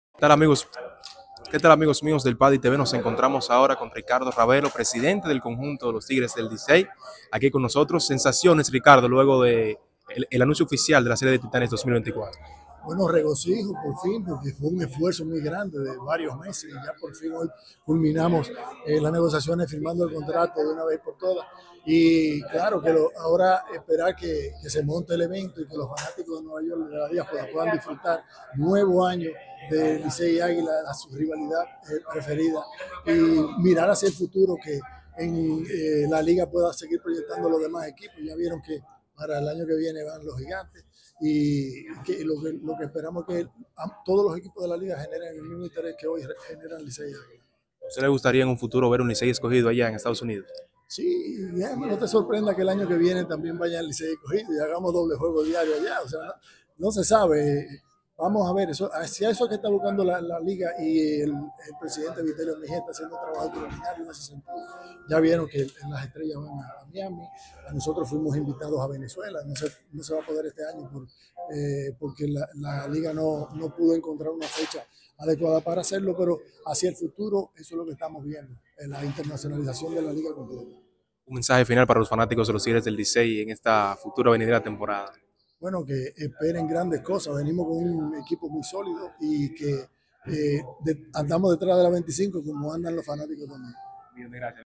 Ricardo Ravelo habla de la serie Titanes del Caribe que será celebrada en el Yankees Stadium
Medio de comunicación deportivo a nivel nacional e internacional con la finalidad de darles el mejor contenido del mundo.